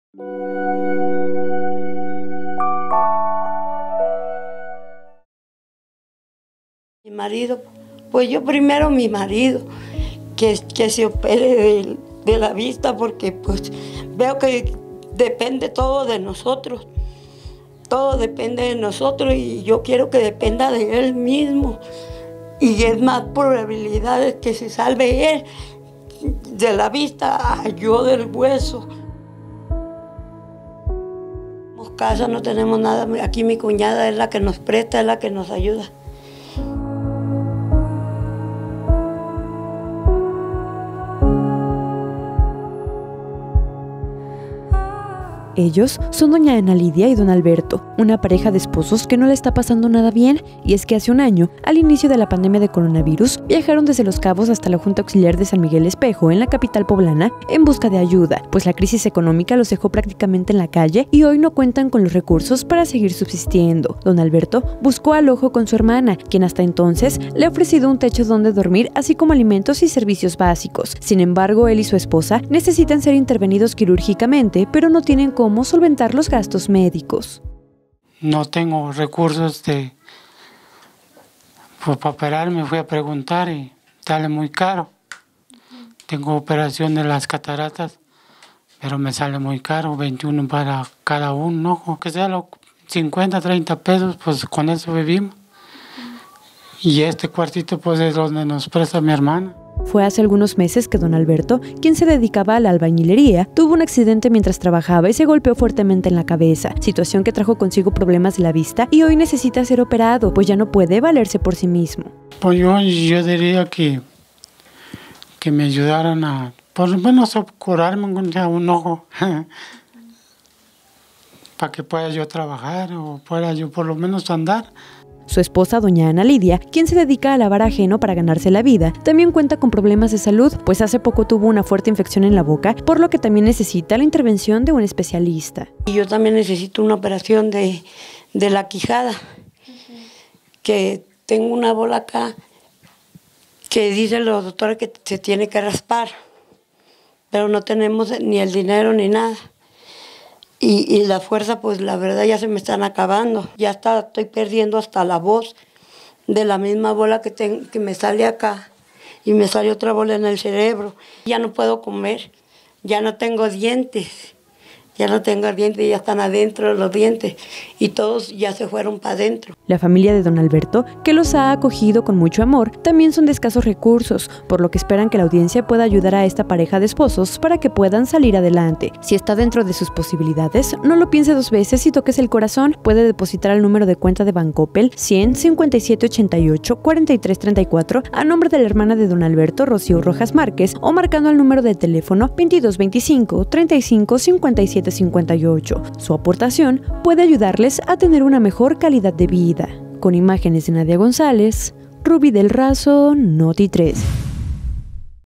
Alberto y Lidia necesitan ser operados con urgencia, pero la pareja no tiene recursos ni para comer.
PueblaSolidaria 🔴 "Por lo menos que me ayuden a curarme un ojo". Alberto tiene cataratas en ambos ojos y a Lidia, su esposa, le tienen que hacer una cirugía en la boca, pero la pareja, en medio de su crisis económica, no tienen ni para comer, Si puedes ayudarlos, llama al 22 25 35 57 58 o deposita Bancoppel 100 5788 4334.
Rubí Del Razo @Canal13Puebla